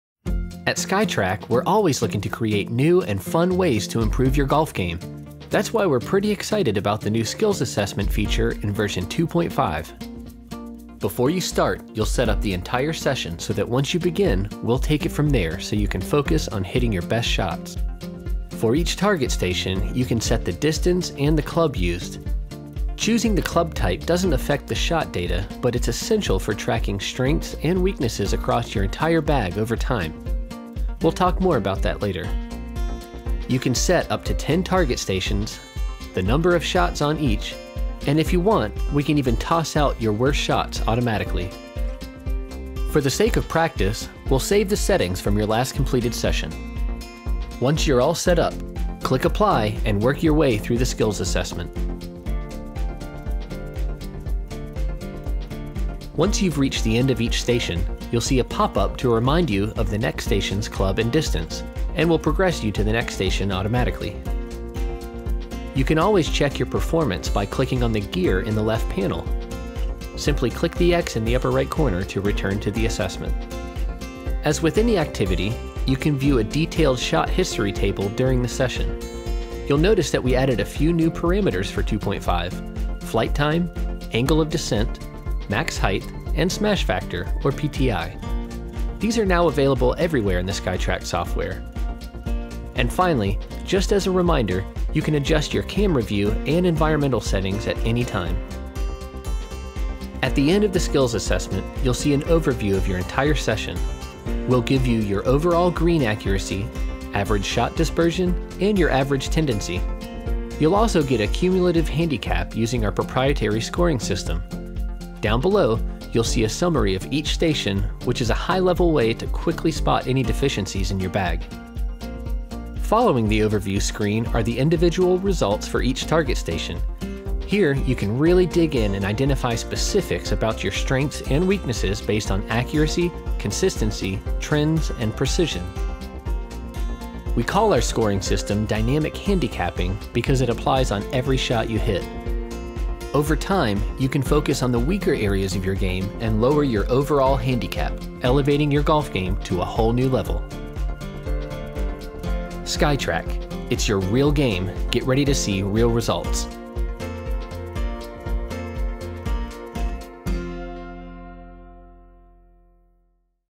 SkyTrak Skills Assessment
Overview of the new Skills Assessment feature in v2.5